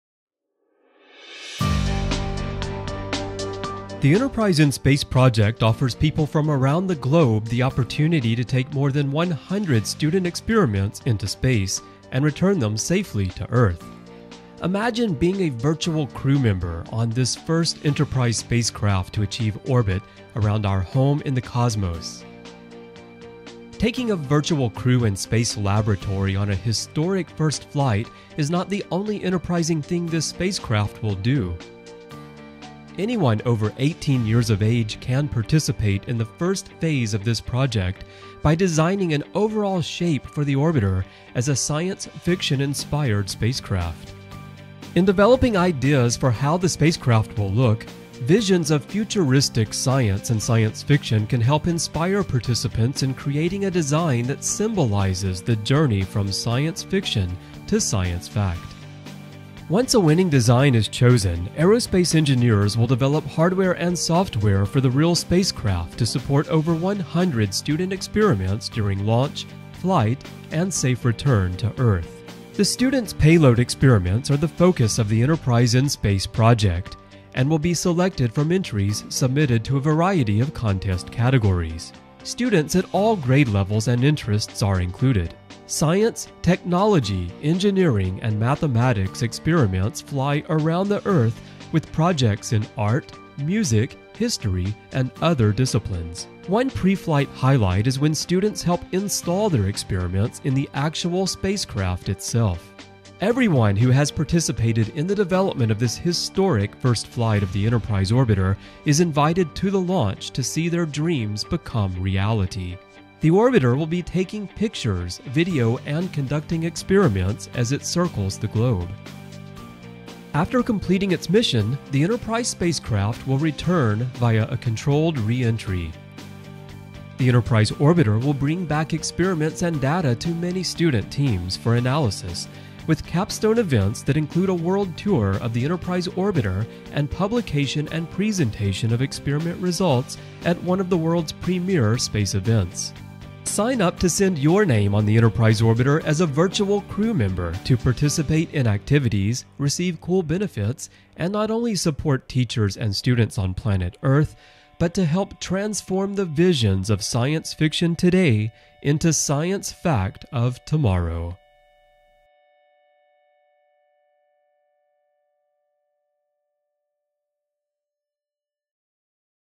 EnterpriseInSpace.org - Fly your name in space
The multi-pronged mission of the Enterprise In Space project is to design, build, fly, and eventually return to earth an orbiter containing student experiments. This project will be a tribute to the many great visionaries of science and science fiction. It will demonstrate and pioneer new technologies while inspiring and encouraging space enterprise. It will promote the development of educational curricula and activities contributing to related future endeavors in science, technology, engineering, arts, and mathematics (STEAM). In general, this project is intended to engage and inspire the next generation – all ages and walks of life – by igniting a renewed interest in space exploration and development.